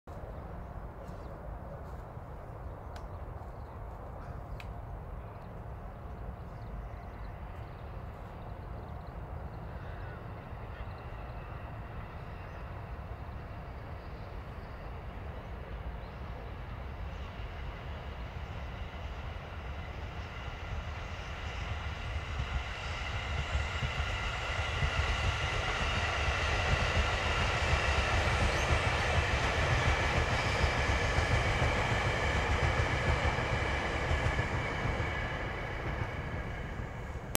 253系1000番台「日光・きぬがわ」用が展示のため回送
東武線直通特急「日光・きぬがわ」用として改造された253系1000番台の6両編成が、品川駅での車両展示のため、朝の東北線を回送されました。